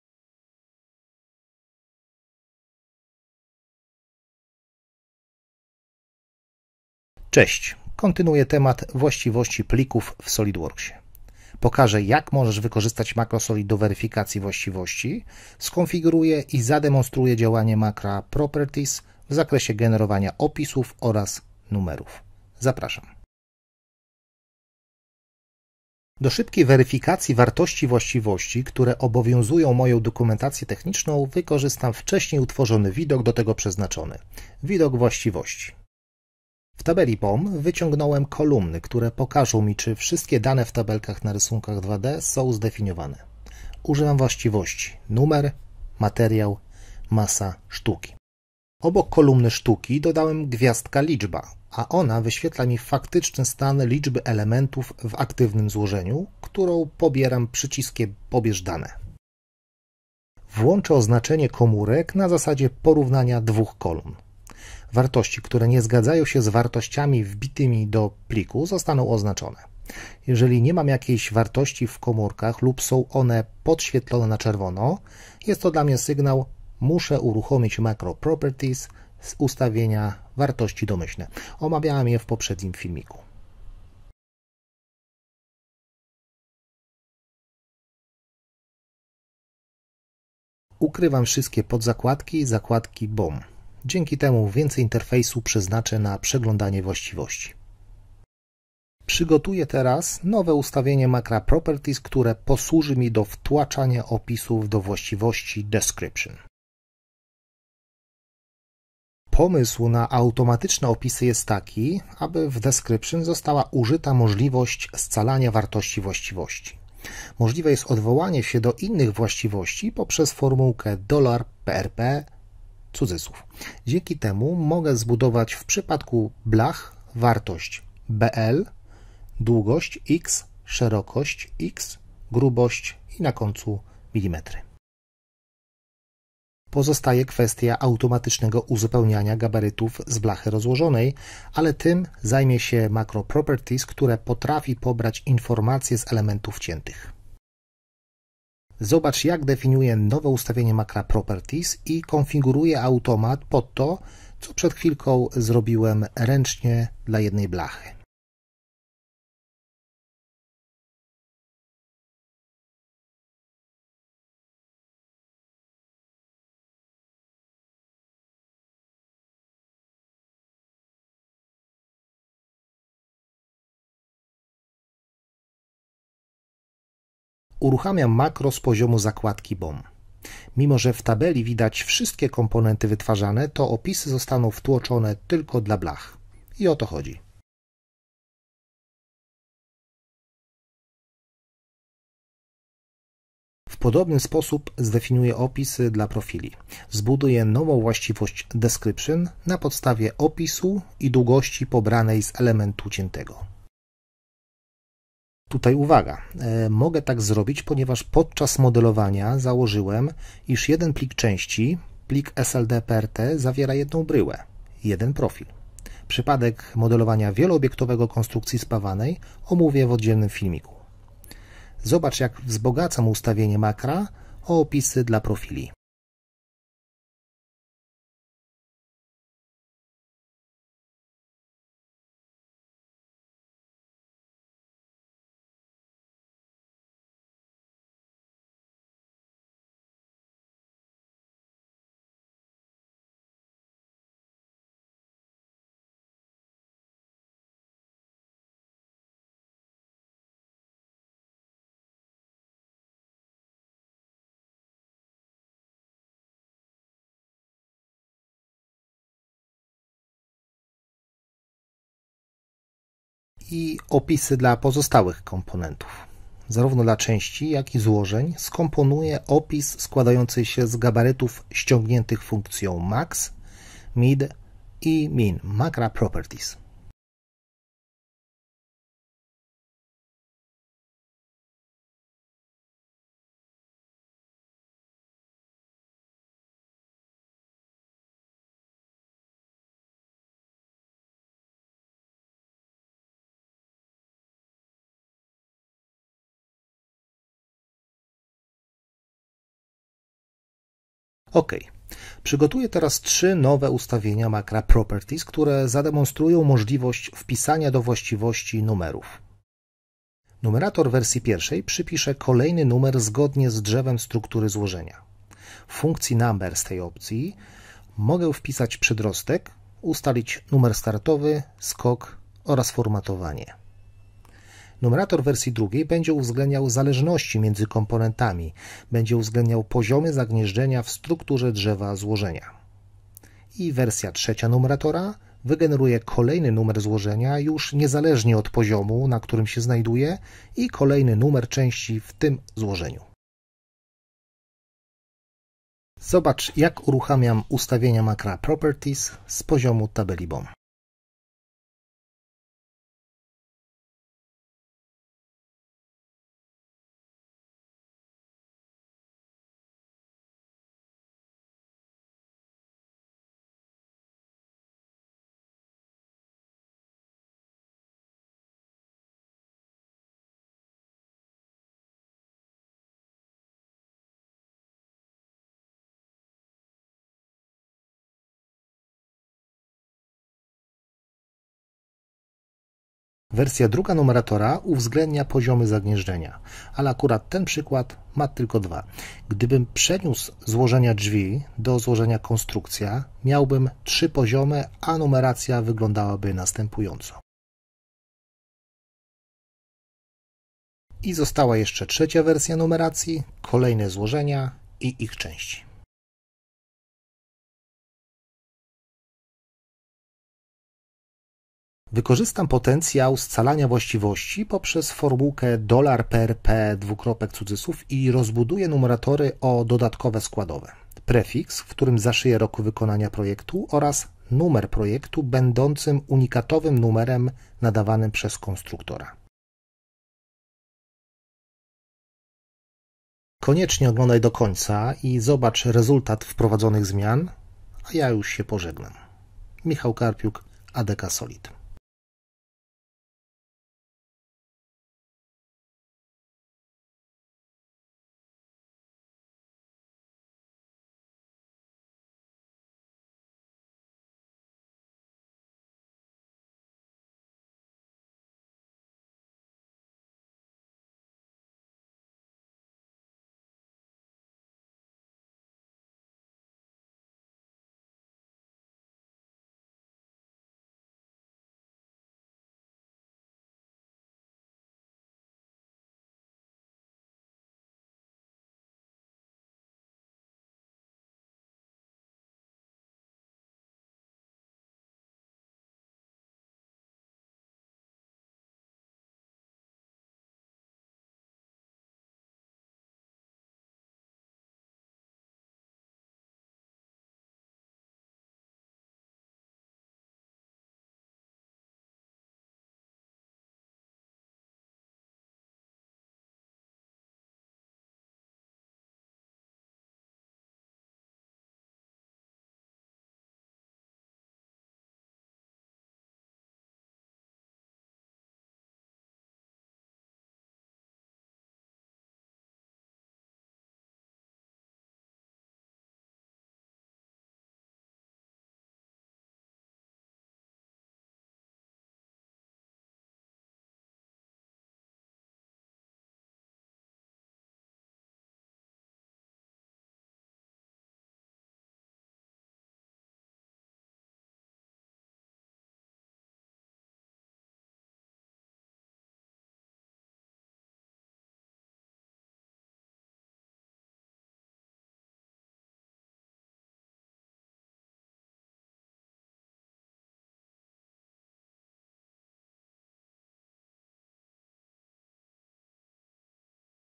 Makro Properties - Description, Numerator
W filmiku pokazano jak możesz wykorzystać MacroSolid do weryfikacji właściwości w SOLIDWORKS. Skonfigurowano i zademonstrowano działanie makra Properties w zakresie generowania opisów oraz numerów.